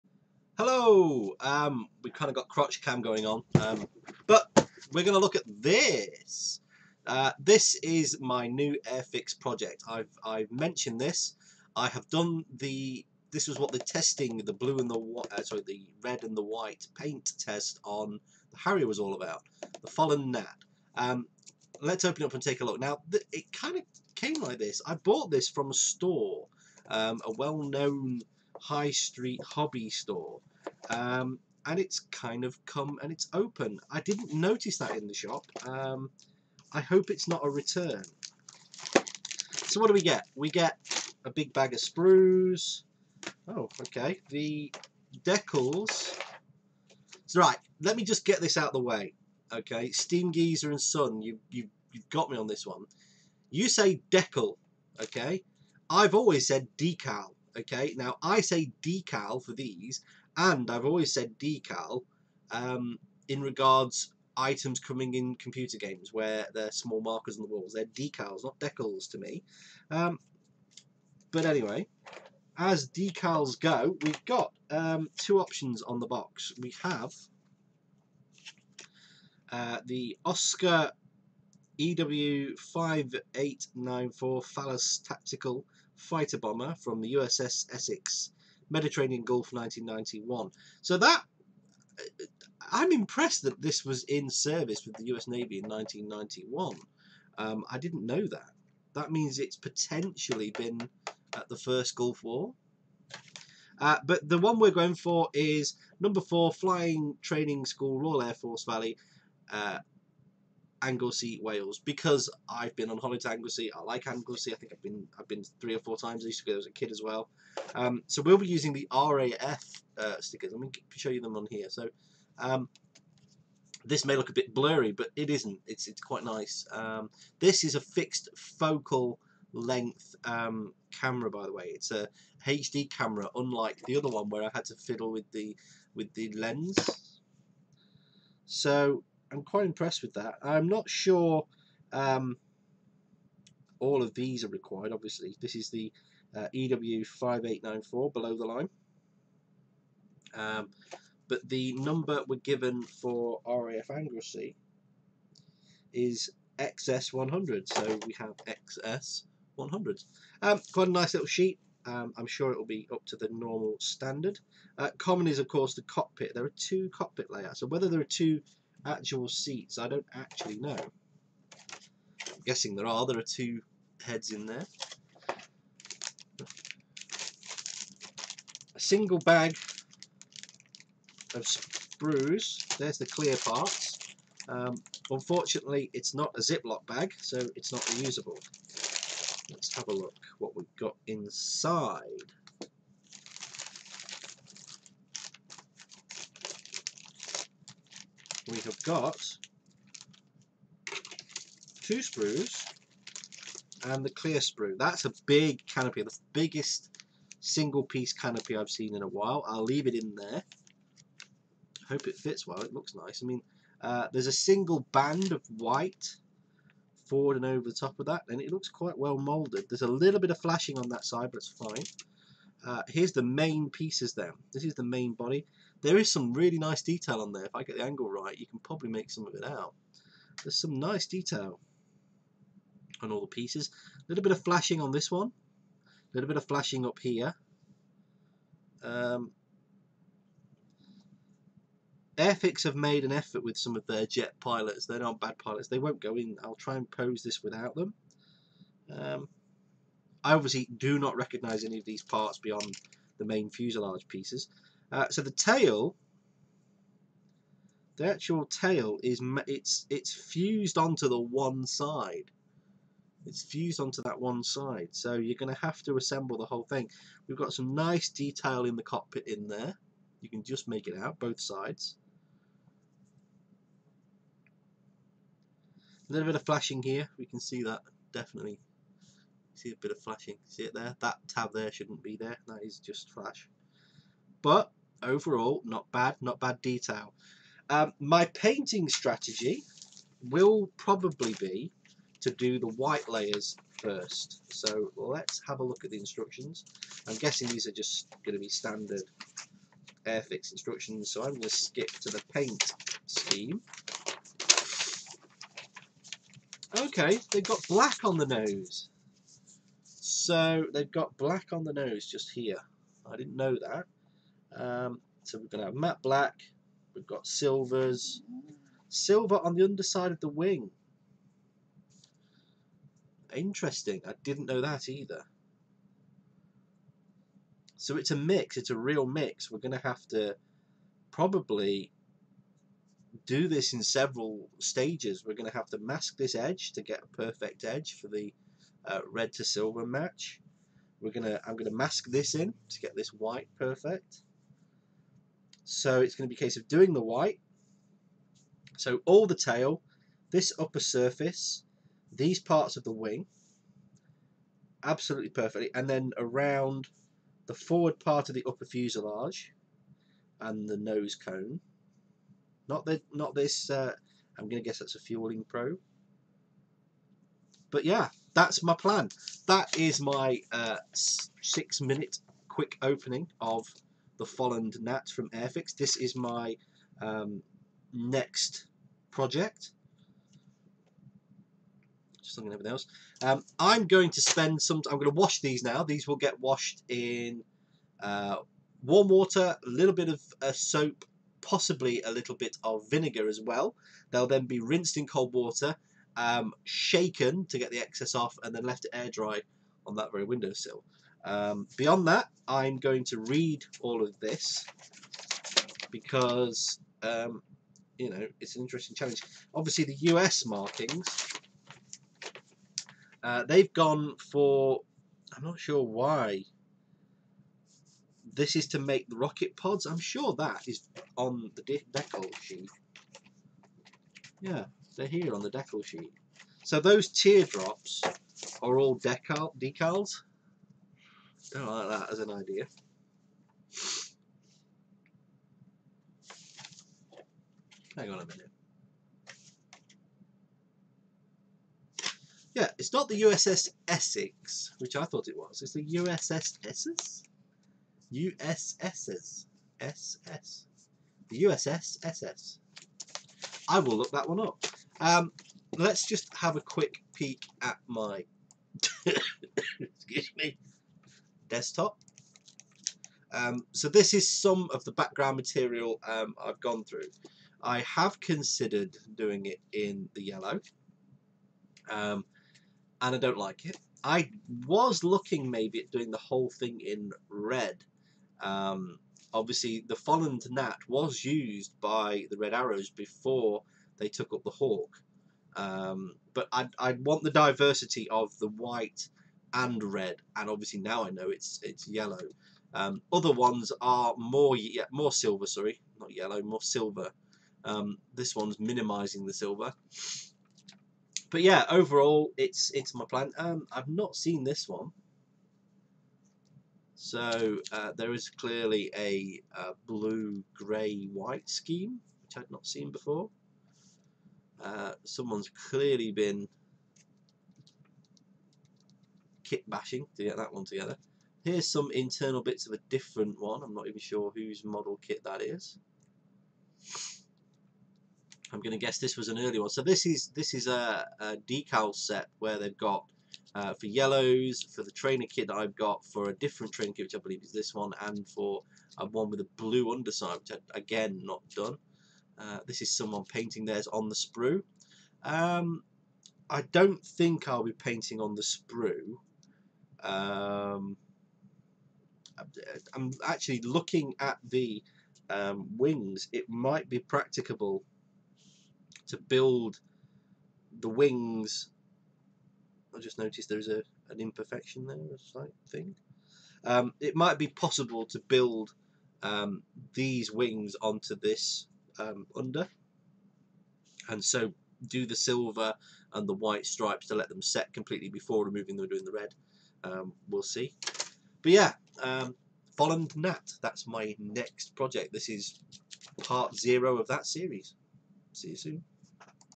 Airfix : Folland Gnat T.1 : 1:72 Build Series - Pt 0 Unboxing
Unboxing and examining my next project the Follant Gnat T1 trainer from Airfix, I'm going to do this in the training colours of RAF Valley on Angelsey.